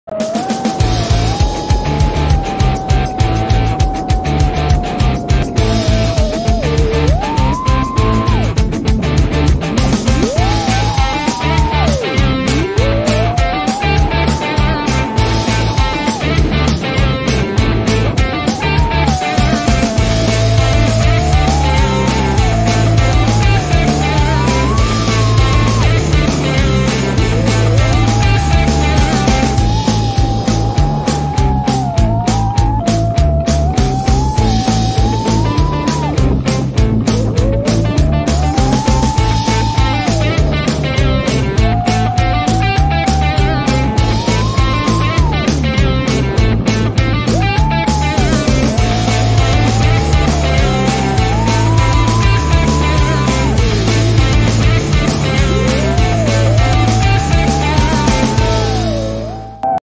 Xing 2 2207 1855kv motors test run on HyperLite Glide 5”
Testing out iFlight Xing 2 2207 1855kv motors
Insta360 Go2, FPV Cycle HyperLite Glide Frame
6s 1300amh CNHL Black series battery , DJI Caddx Vista
HGLRC FCF760 & 60 amp ESC